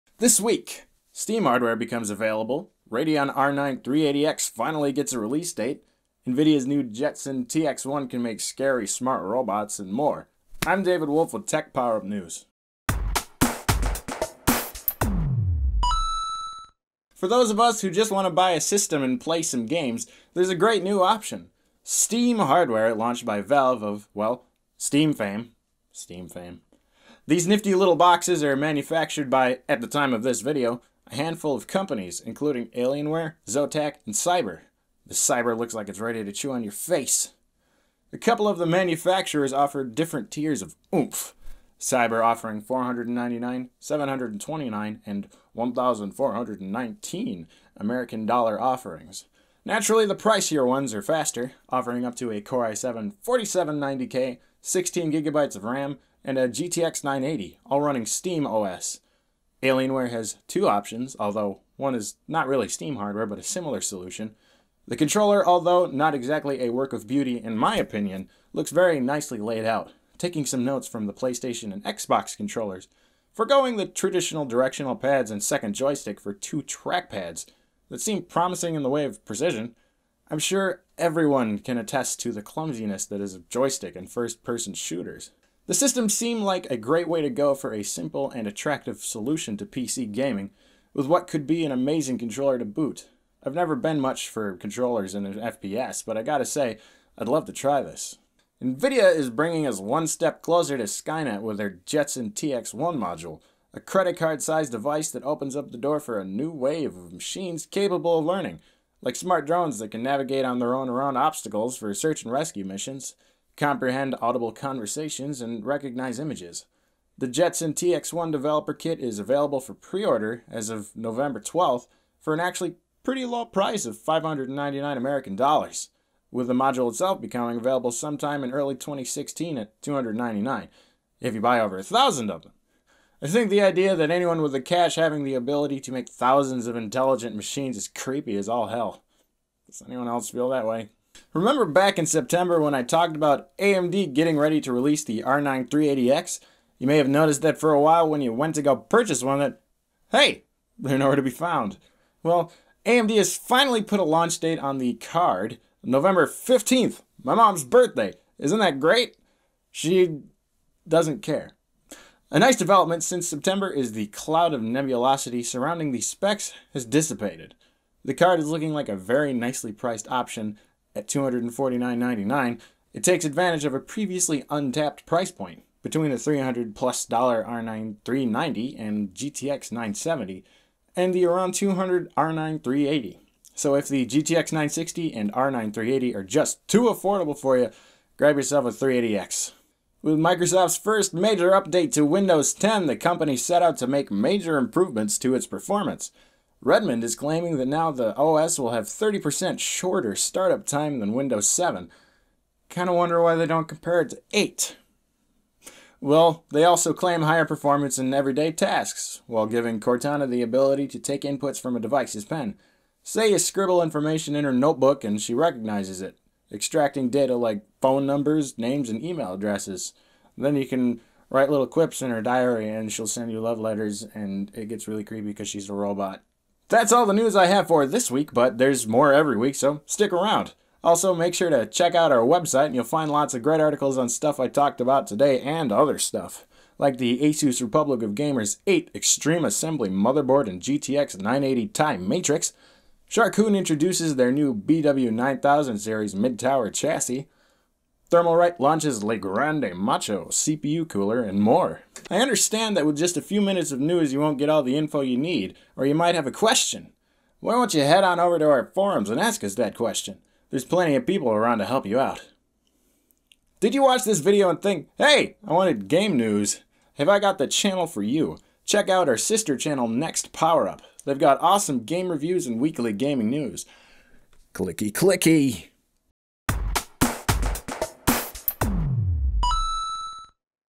TechPowerUp News : Steam Machines, Jetson TX1 by Nvidia, Radeon 380X
Here are the articles I used!

NVIDIA Announces Jetson TX1 Module to Bring Deep Learning to Robots and Drones :

Wanna read more tech news? Hit up